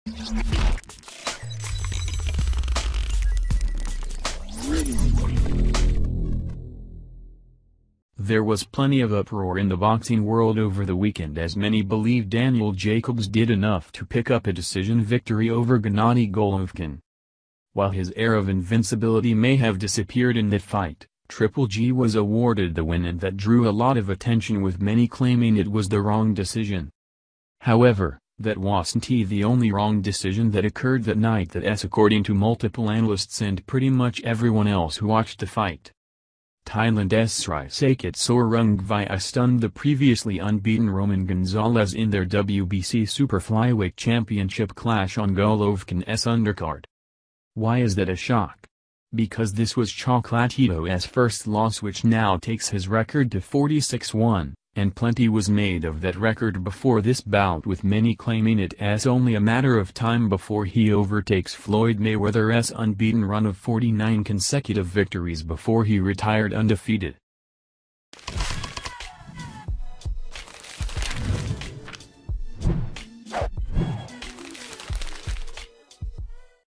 The best pound for pound boxer in the world just lost his first ever fight
There was plenty of uproar in the boxing world over the weekend as many believed Daniel Jacobs did enough to pick up a decision victory over Gennady Golovkin.